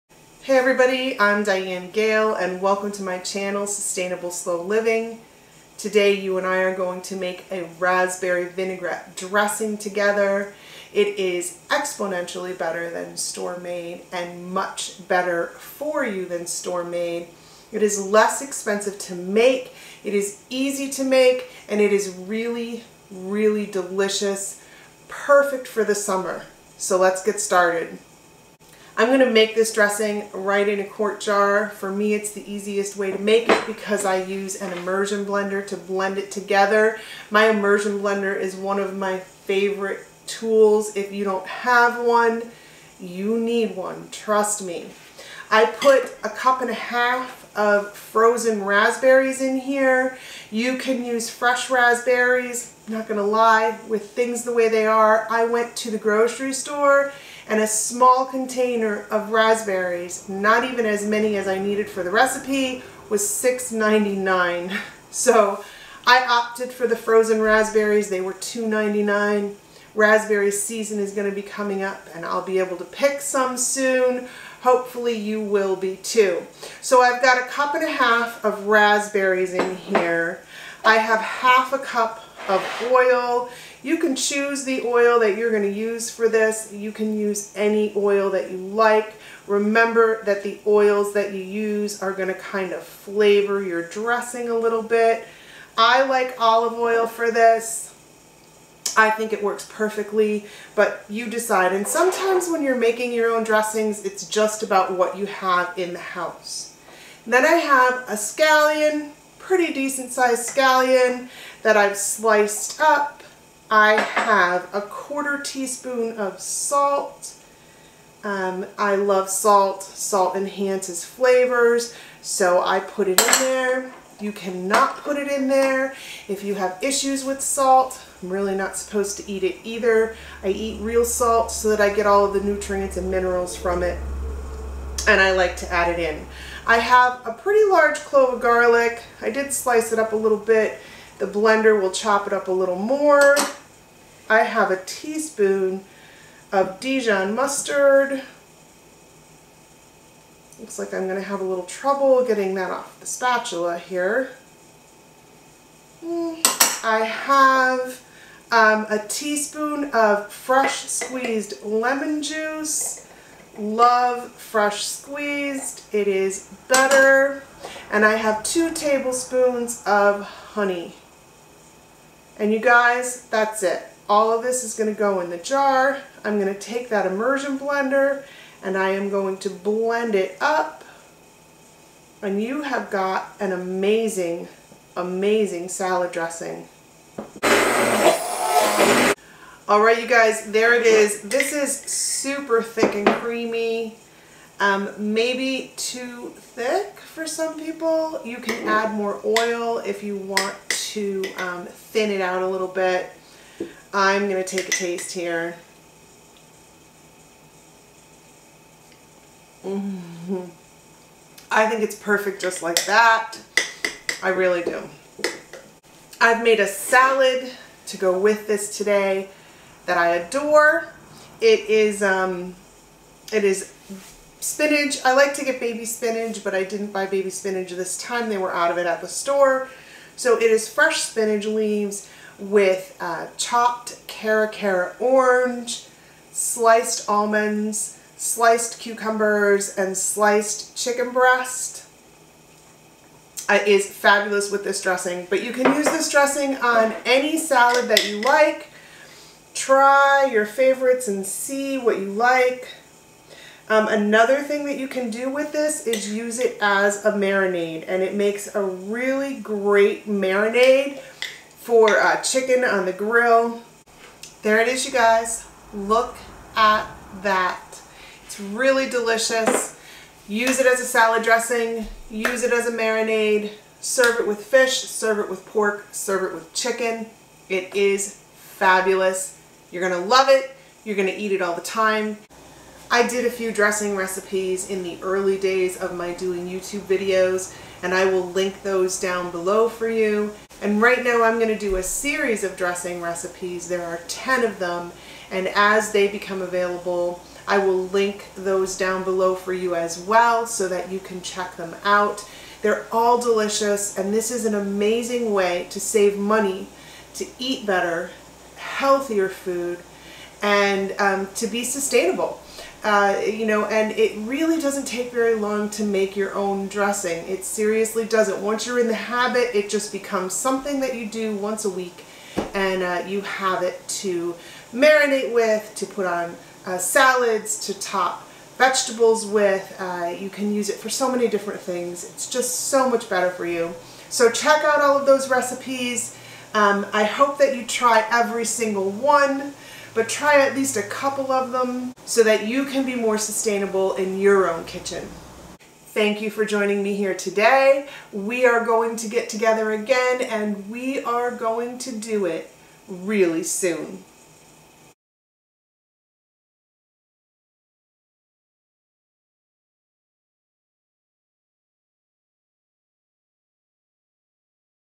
Make This Raspberry Vinaigrette Dressing Recipe [Made With REAL RASPBERRIES]
This raspberry vinaigrette dressing recipe is quick and easy to make. And it's like putting summer in a bottle!

It's so much healthier than store bought versions. It's thicker and creamier too.

You can use it on all your favorite summer salads. Or you can brush it over fish, chicken, or pork on the grill. It's DELICIOUS either way.

So ... let me show you how to make fresh raspberry vinaigrette dressing today!